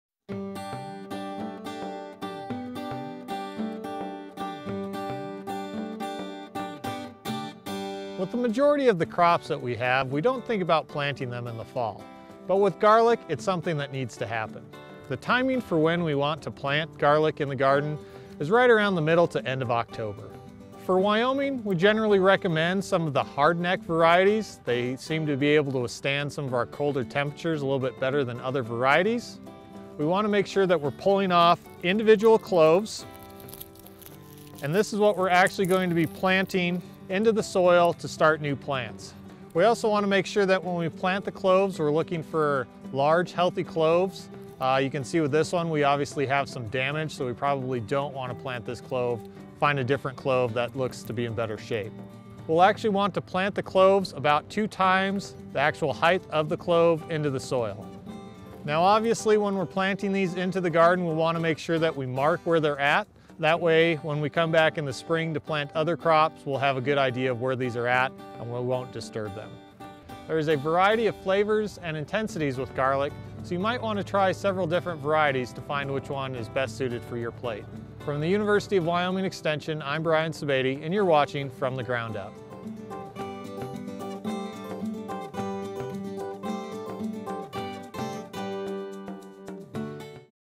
Planting Garlic | From the Ground Up
Garlic is a fall planted crop that is harvested in midsummer. Try a variety of flavor and intensity of hardneck garlic in your garden.